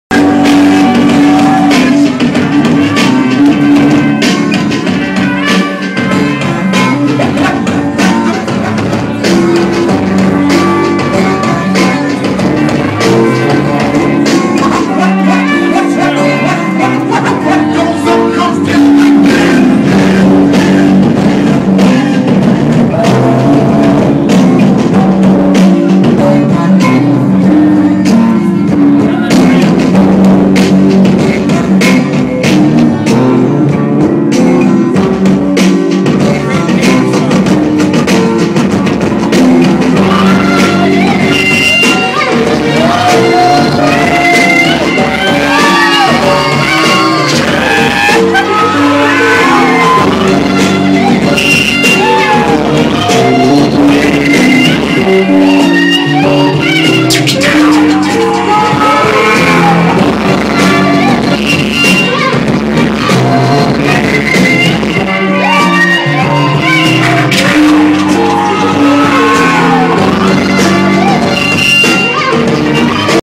Blockhead & Dj Signify Live in Seattle
11/12/09.   I had the wonderful opportunity of opening for Blockhead, so I got video of it.